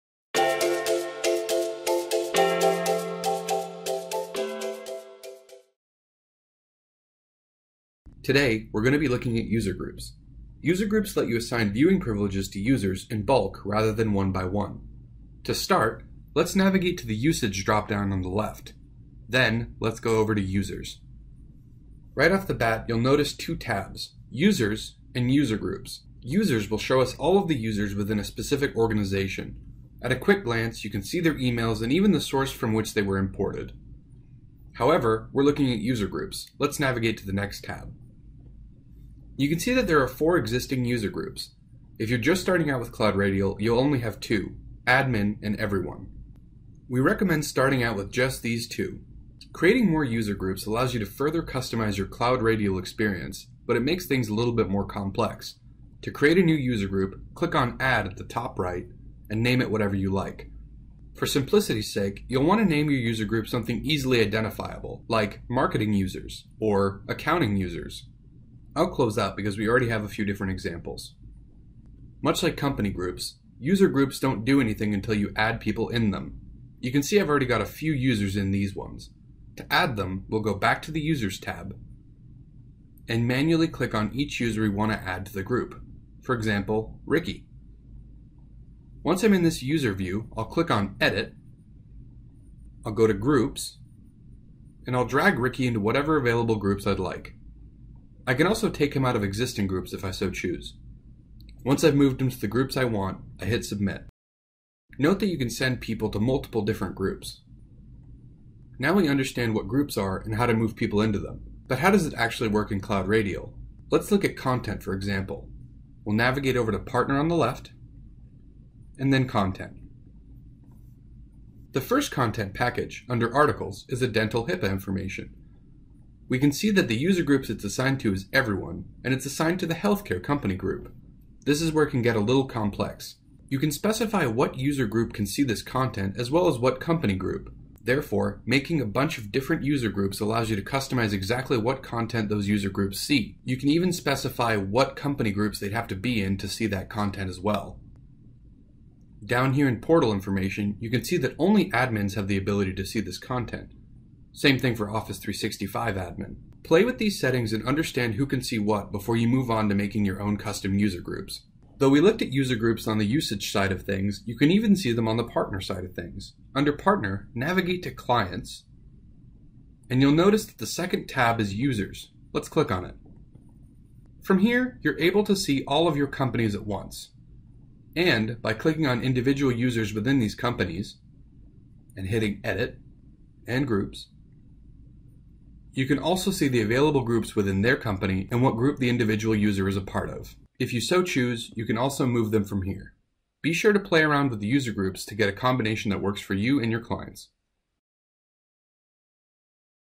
Working with User Groups in CloudRadial
User groups help you organize your clients by type. With them, you can quickly modify the content provisioning to only be seen by certain category of user.

Watch this video to understand how to create a user group, how to assign users to it, and what it can do from a content perspective.